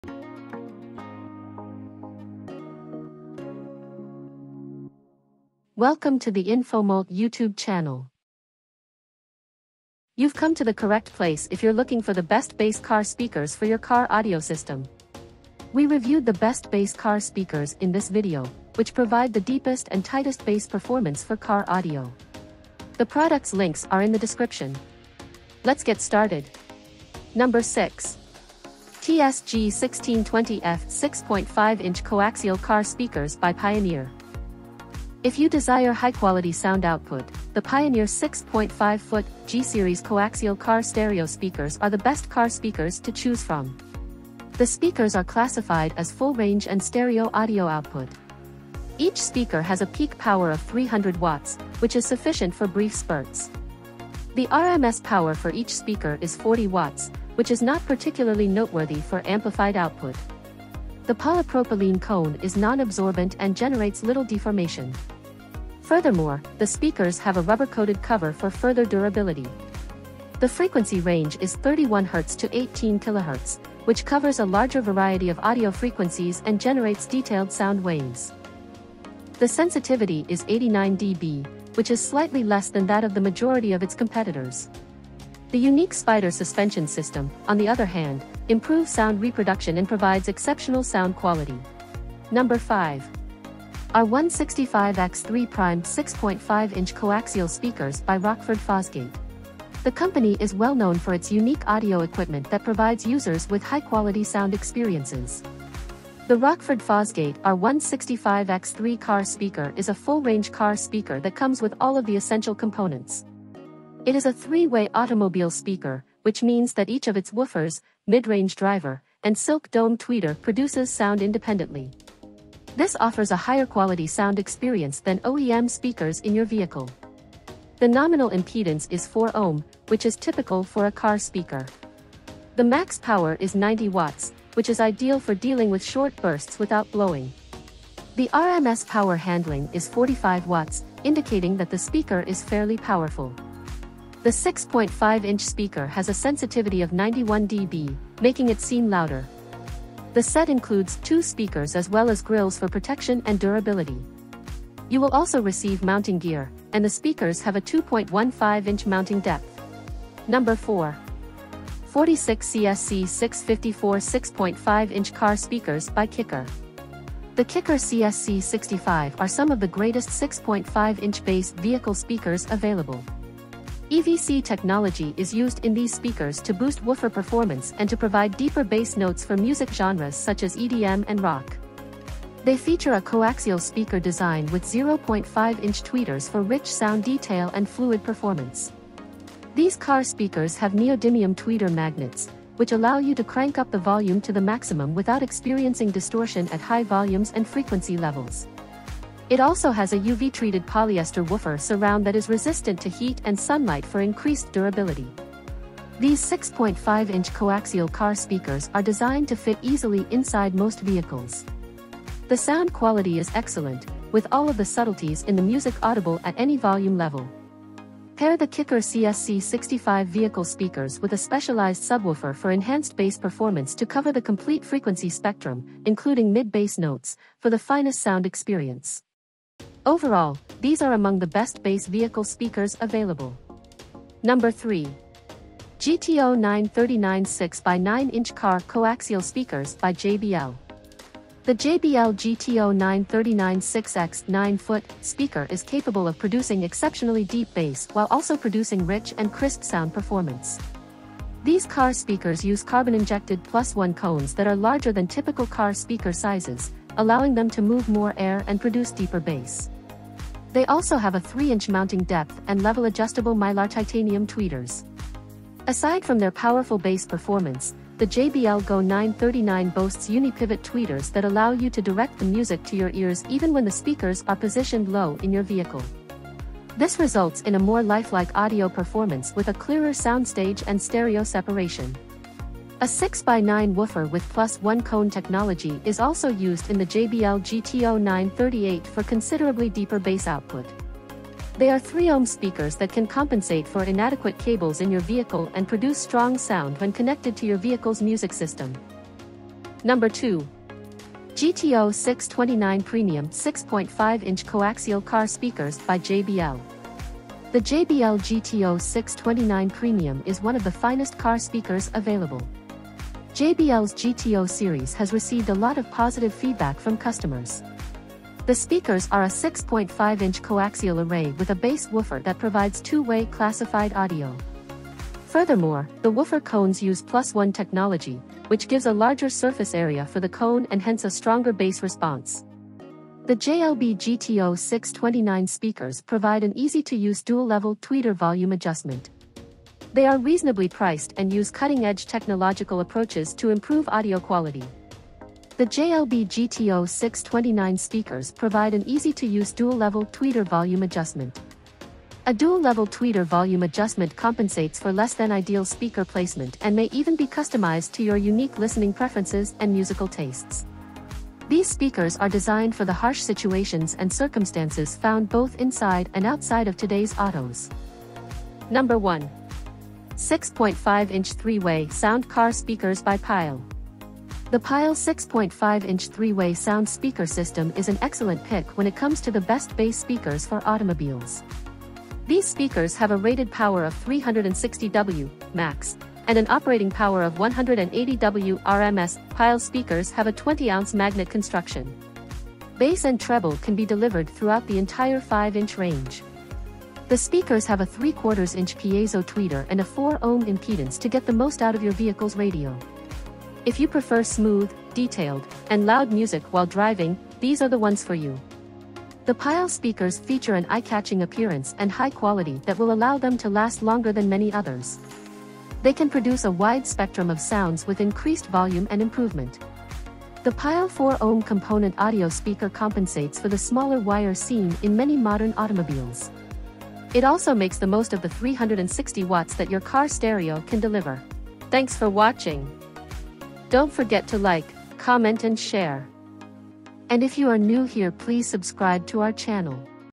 👉 Best Car Speakers for Bass and Sound Quality in 2023 | Infomult.com
Welcome to our video on the top 6 best car speakers for bass and sound quality. If you're looking to upgrade your car audio system, having the right speakers can make a huge difference in your listening experience. These speakers are specifically designed to fit in most car models and offer a variety of features that can enhance your music listening. In this video, we'll be taking a look at the top 5 car speakers that offer the best combination of sound quality, durability and compatibility. Whether you're a music lover or a audiophile, these speakers will help you take your car audio to the next level. So, sit back, relax, and let's check out the best best car speakers for bass and sound quality.

No.1
R165X3 Prime 6.5" 3-Way Coaxial Speakers by Rockford Fosgate

No.2
GTO629 Premium 6.5-Inch Co-Axial Car Speakers by JBL

No.3
6.5" Three-Way Sound Car Speakers by Pyle

NO.4
GTO939 6x9" 3 Way Black Car Coaxial Speakers by JBL

No.5
46CSC654 6.5’’ Coaxial Car Speakers by Kicker

No.6
TS-G1620F 6.5’’ 2-Way Coaxial Car Speakers by Pioneer